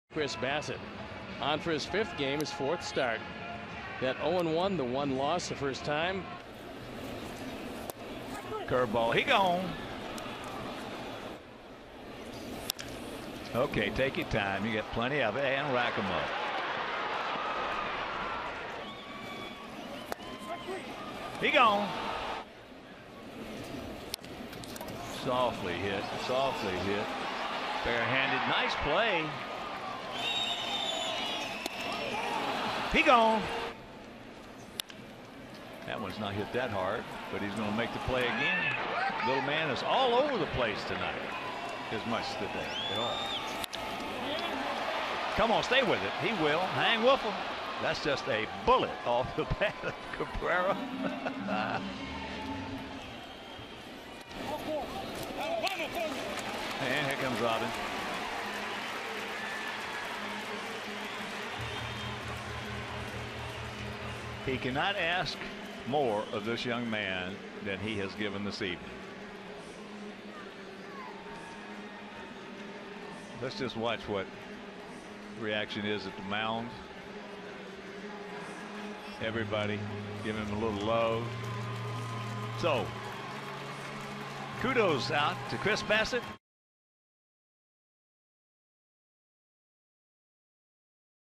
CWS@DET: Bassitt pitches 7 2/3 scoreless innings
9/22/14: Chris Bassitt dominates the Tigers over 7 2/3 frames of shutout ball, allowing six hits and striking out three in his first win